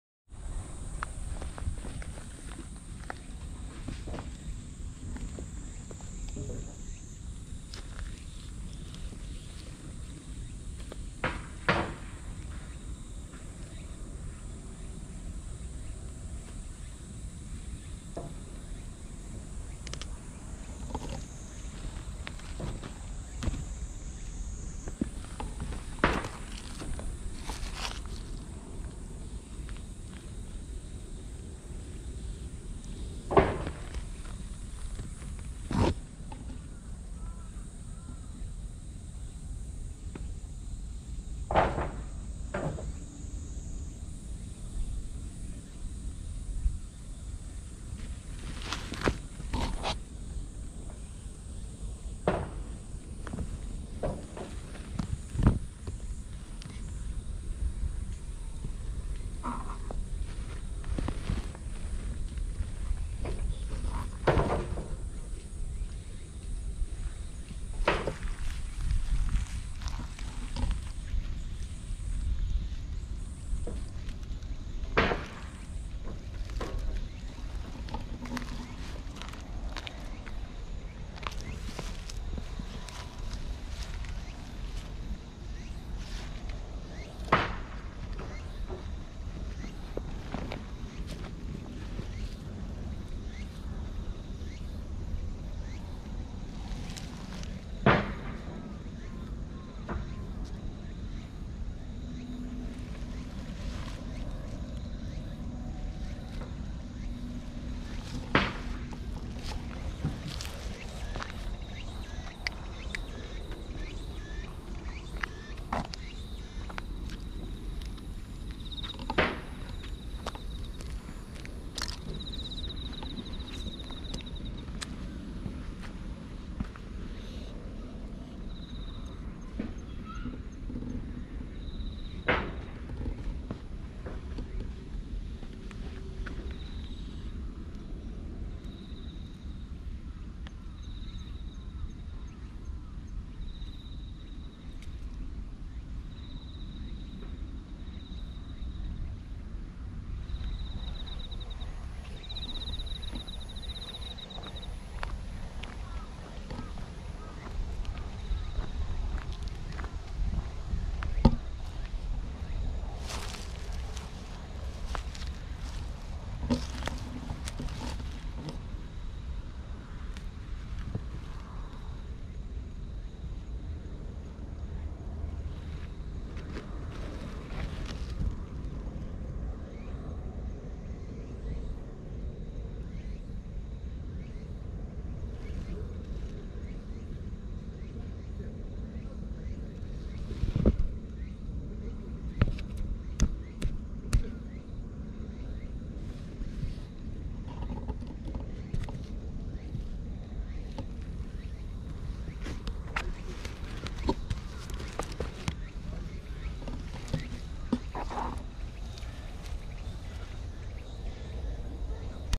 友達ができたぼっち猫！？マーブル黒猫と三毛猫がかくれんぼをして遊ぶ
防波堤にいた三毛猫をナデナデするとお尻を高々と上げてカワイイ